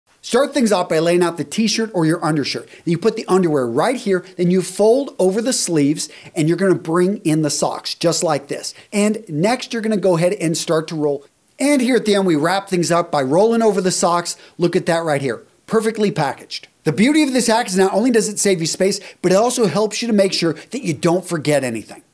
Pack Like A PRO // The Skivvy Roll
An even more convenient packing method is called the Skivvy Roll. The skivvy roll technique is also called the “grunt roll” and is very similar to the “ranger roll”. It is a great way of packing!

📫 Shipping Address

Antonio Centeno
USA

PRODUCTS

RECOMMENDED PRODUCTS